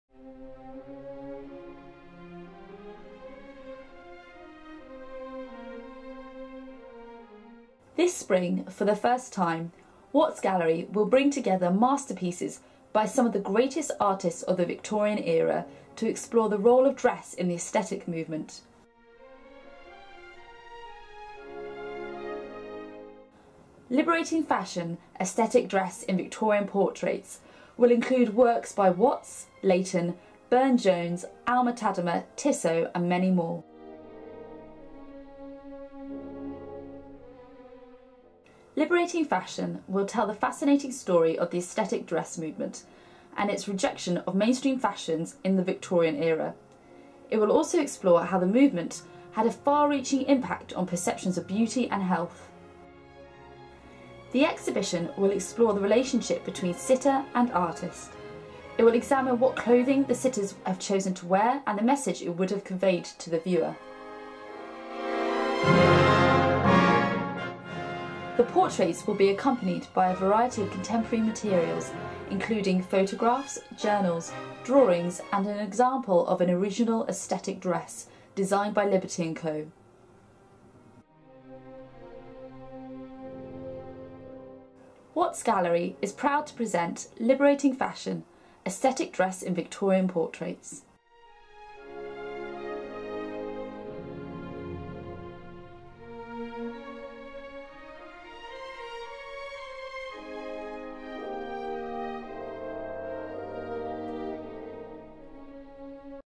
Liberating Fashion: Aesthetic Dress in Victorian Portraits
This spring, Watts Gallery will bring together masterpieces by some of the greatest artists of the Victorian age to explore the Aesthetic Dress Movement – a movement pioneered by artists that rejected Victorian mainstream fashion with its tiny corseted waists and cumbersome bustles, favouring instead flowing, draped styles that enhanced the natural beauty of the female form.

The exhibition will feature exquisite portraits by, amongst others, G F Watts, Frederic Leighton, Edward Burne-Jones, Lawrence Alma-Tadema and James Tissot.